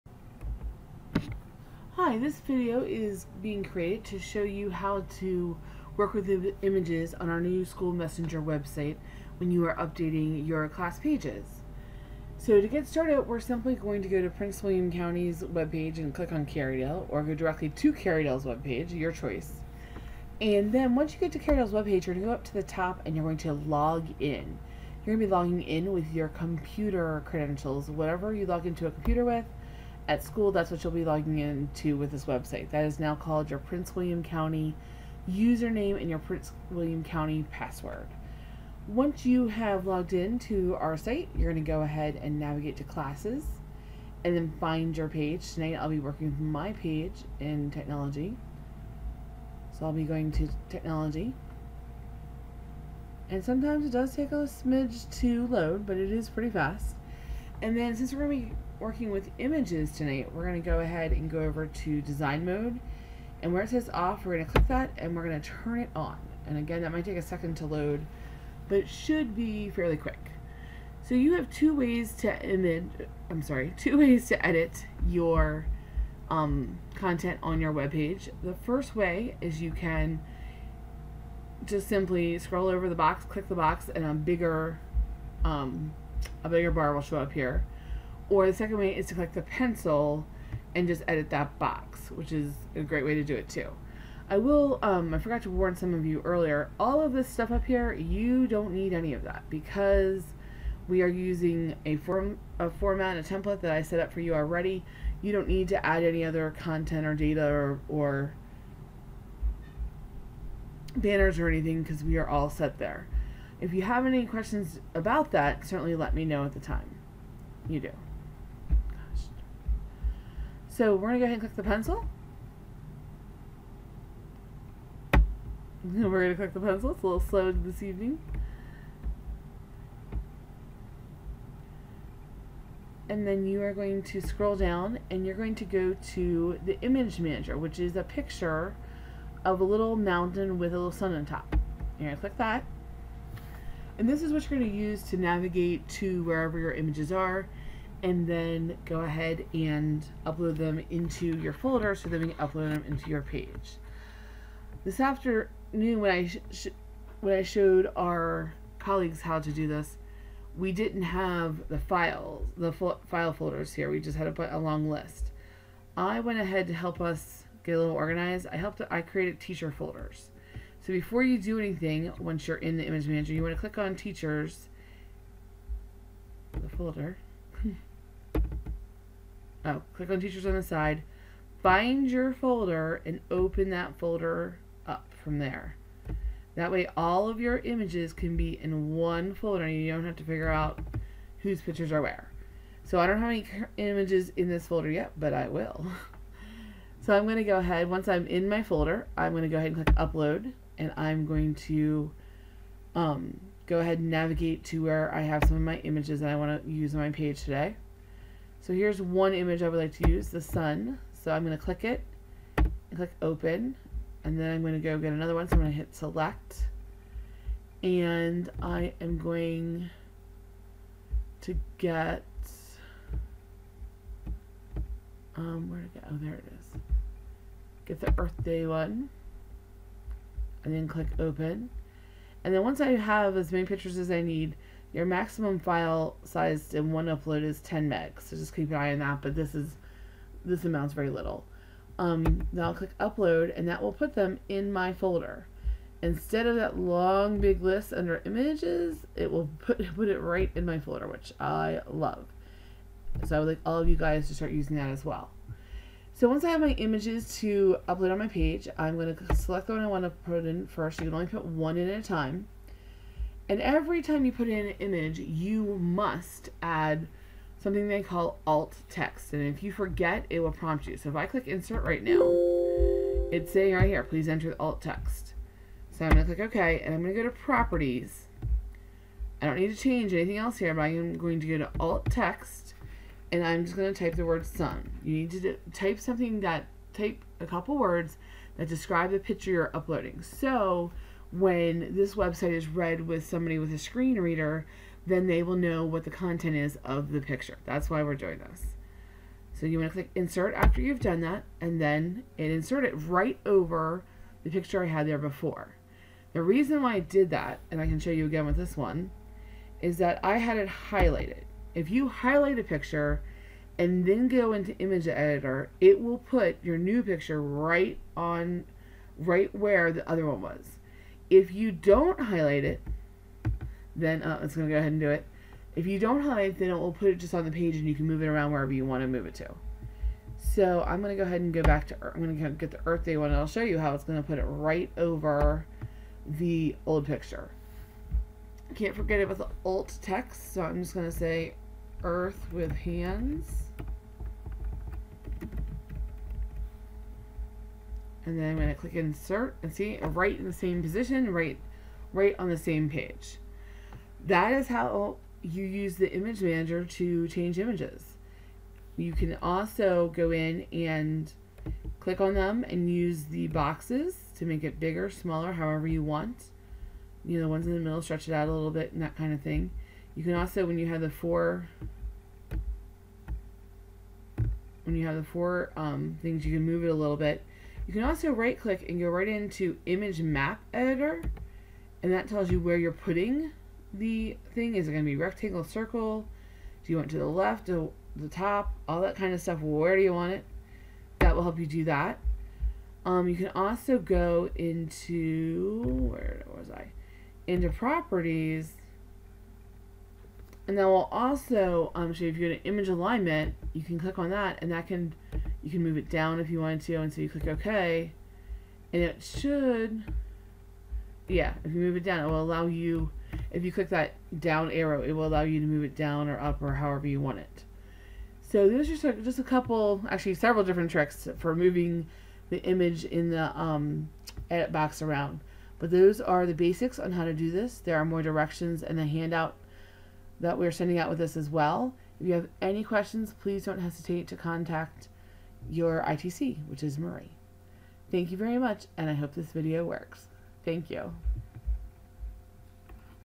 Add Edit Images in School Messenger
Shows how to add and edit images in school messenger.  created by M. Ermold